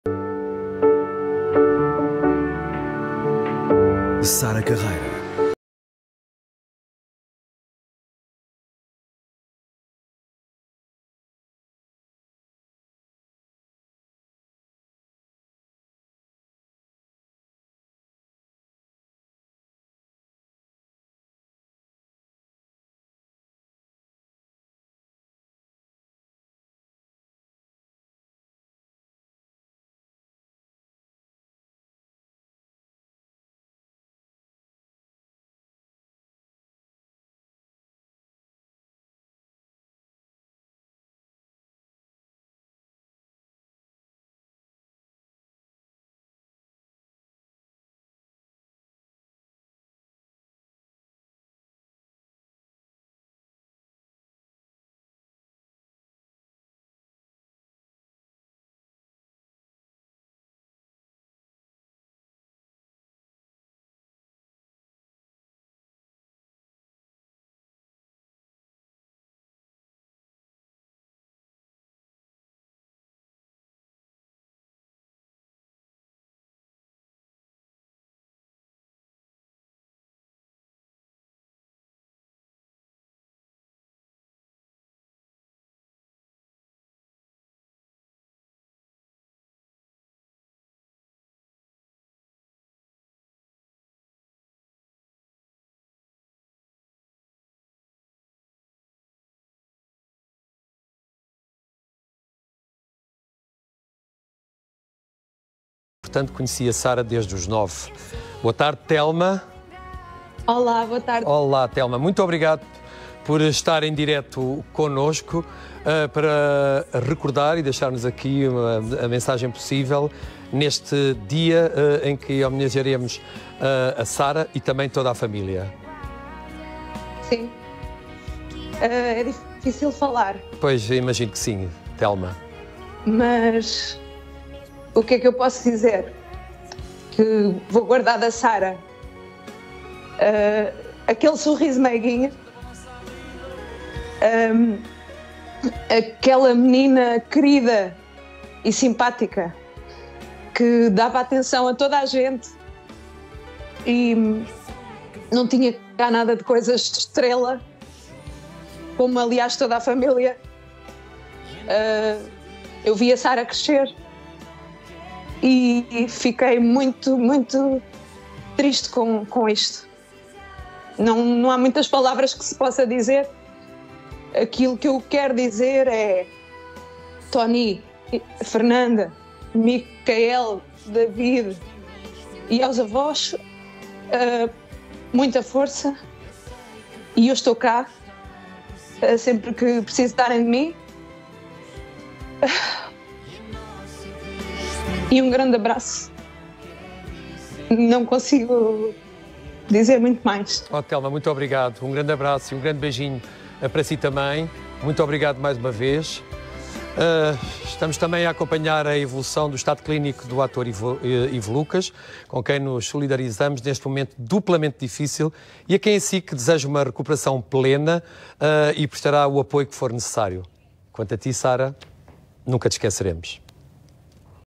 Morte de Sara Carreira: Amiga e colega de Tony Carreira recorda a "princesa" da família Carreira
Telma Botelho, amiga da família e colega de trabalho de Tony Carreira, prestou uma homenagem pública a Sara Carreira, que conhece desde sempre, numa emissão especial da SIC.
Imagens: SIC